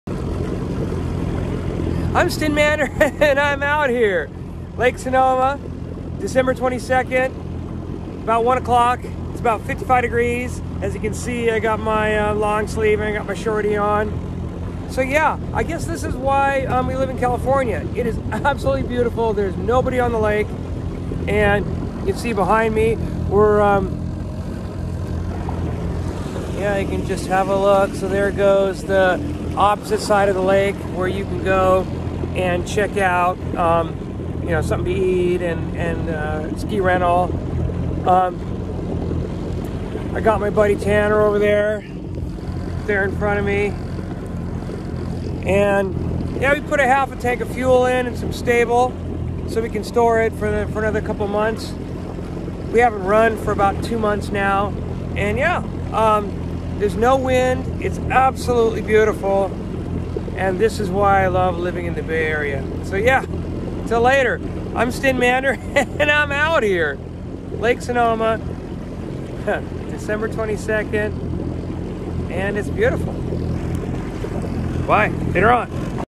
January 8, 2021
We went jet skiing on Lake Sonoma in Northern California, near the city of Napa, 2 days before Christmas 2020 and man was it a cold and bright day!